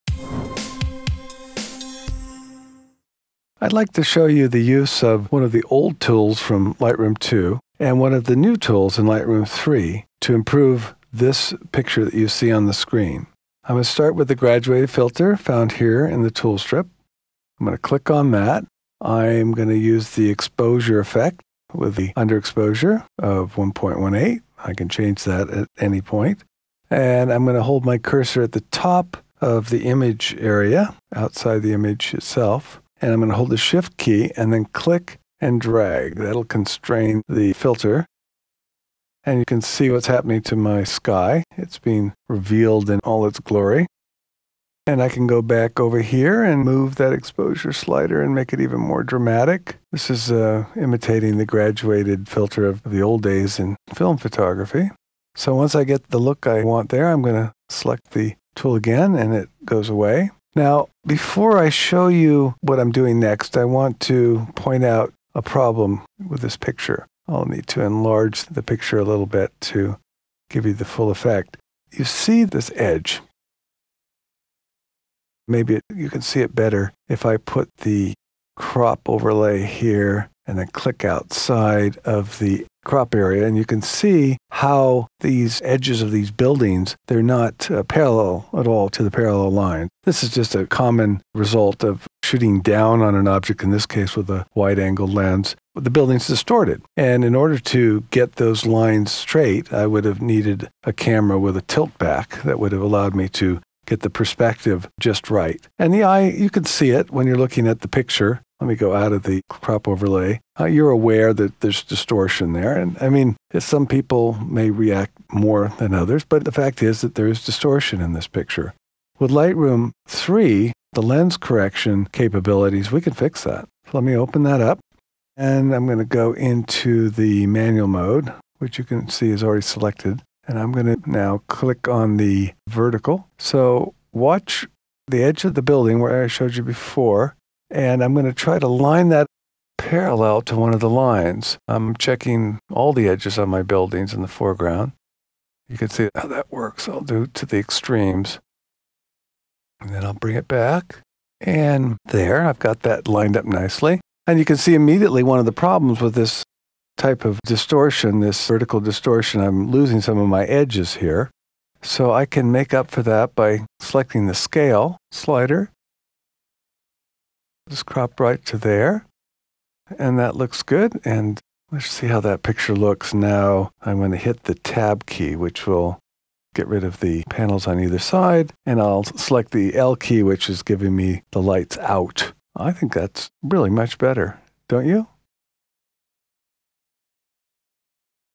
Lightroom 3 - Using an Old Tool and a New One to Improve an Image
In this jump-start you'll see how easy it is to use the Graduated filter and the new Lightroom lens  correction options to improve an image.